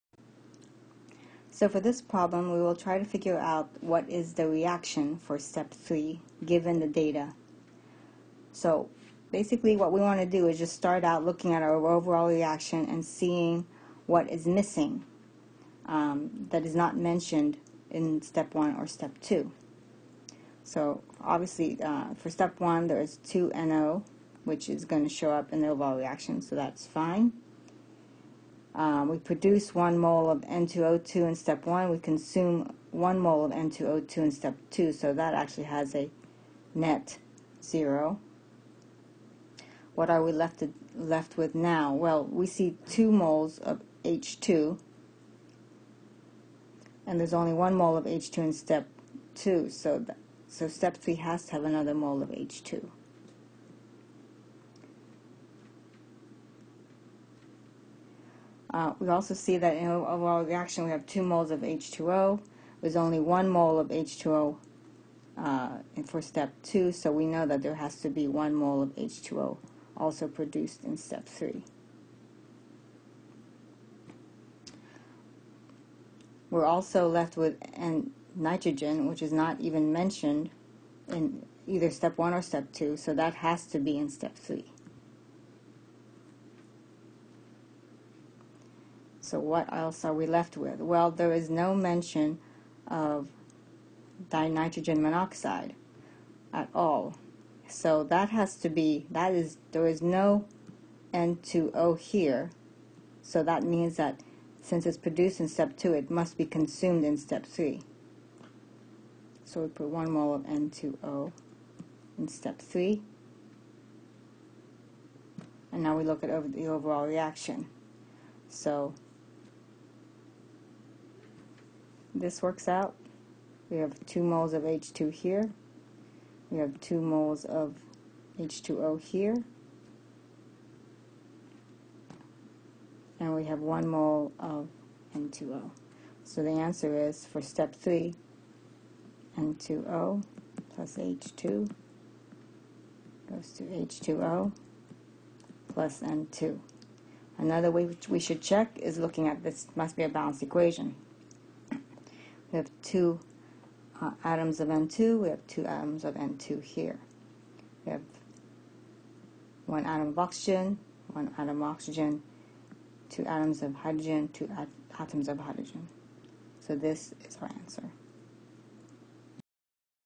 Kinetics Adding Elementary Steps in a Mechanism
From the overall reaction, finding out the third elementary step when the first and second elementary steps are given. Practice in adding reactions to give the net overall reaction.